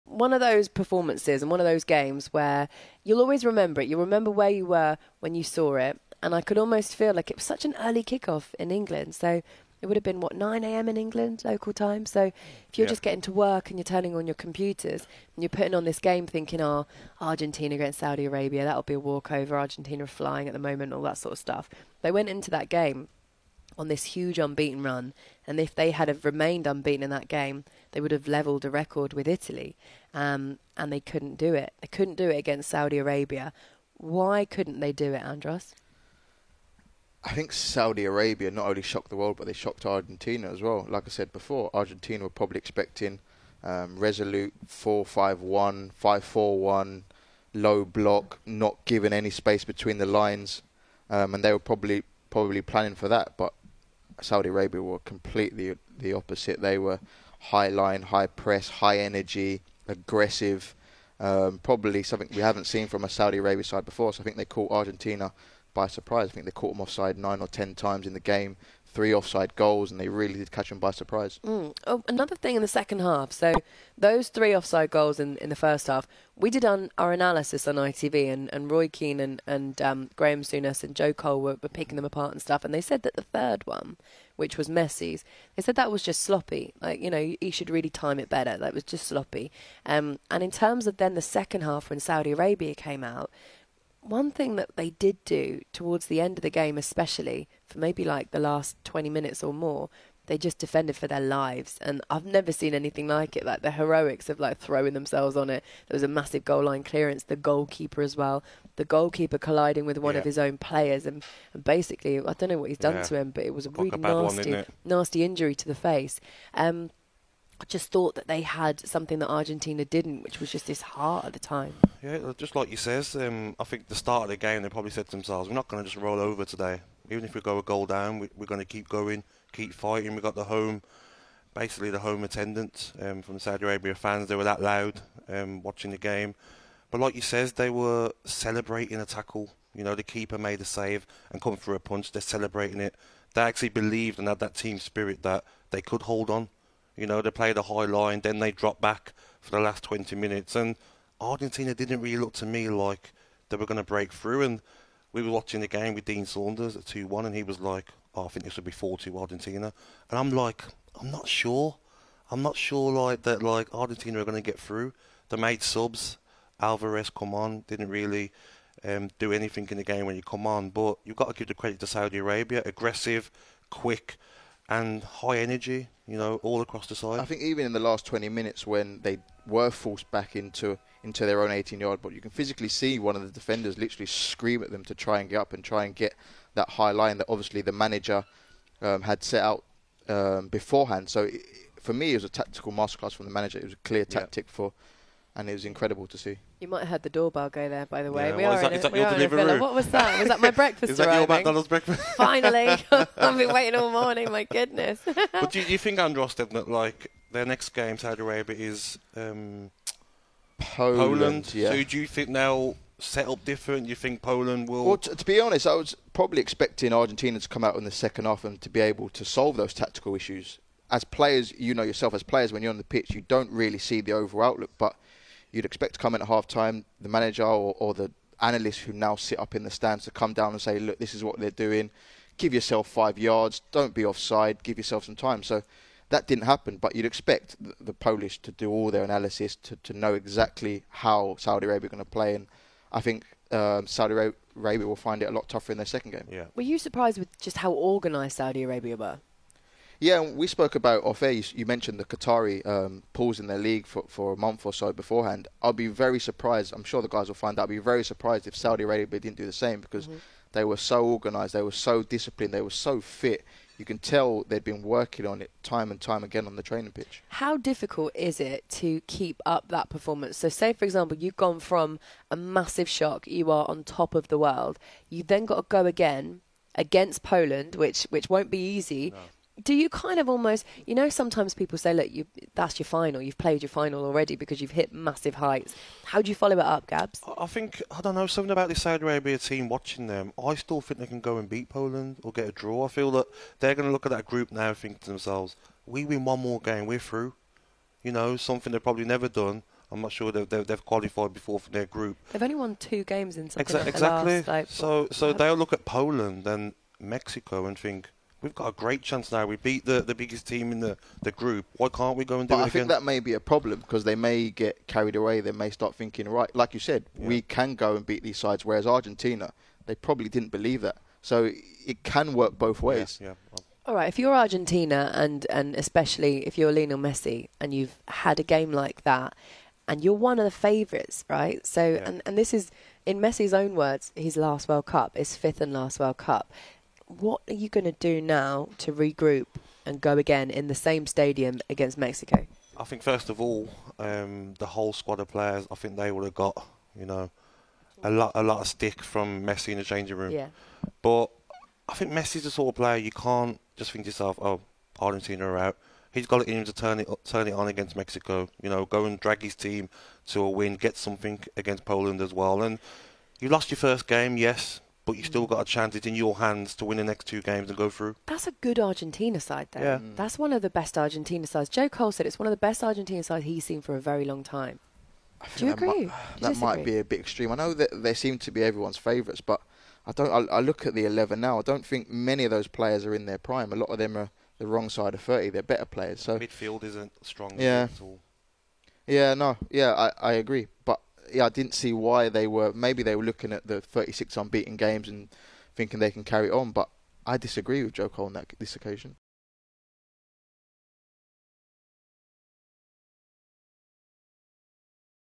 SAUDI 2-1 ARGENTINA 🔥⚽ Andros Townsend & Gabby Agbonlahor react to Argentina's SHOCK World Cup loss
Saudi Arabia beat Lionel Messi's Argentina 2-1 yesterday in their first game of the 2022 FIFA World Cup. talkSPORT's Laura Woods, Gabby Agbonlahor and Andros Townsend discuss!